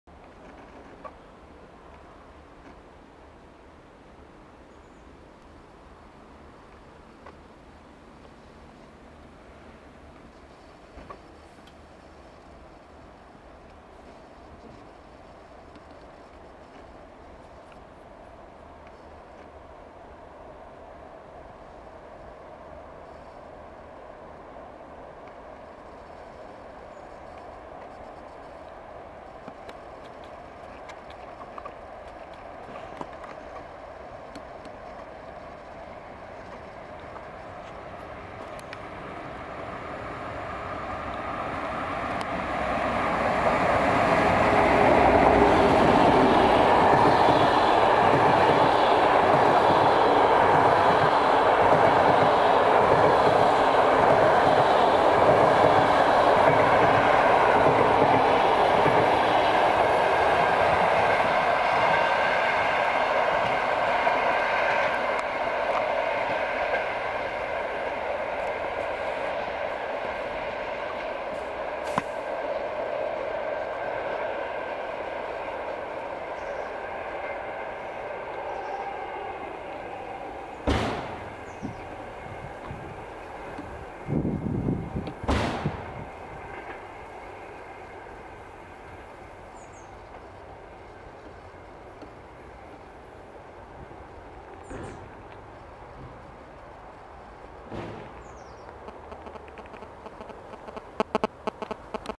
Objazdowy i opóźniony ponad 200 minut TLK "Gałczyński"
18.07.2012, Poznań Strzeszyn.
Piękna warszawska maszyna prowadzi opóźniony o 205 minut pociąg TLK 82512 "Gałczyński" ze Świnoujścia Portu do Lublina mijając perony stacji Poznań Strzeszyn.
Objazd składu spowodowany wypadkiem na stacji Wronki. Co ciekawe skład został wyprzedzony przez TLK "Sukiennice", który planowo przyjeżdża do Poznania blisko półtorej godziny później.
Przepraszam za ruchy obrazu, ale poluzował mi się statyw ;p
Pozdrowienia dla mechanika !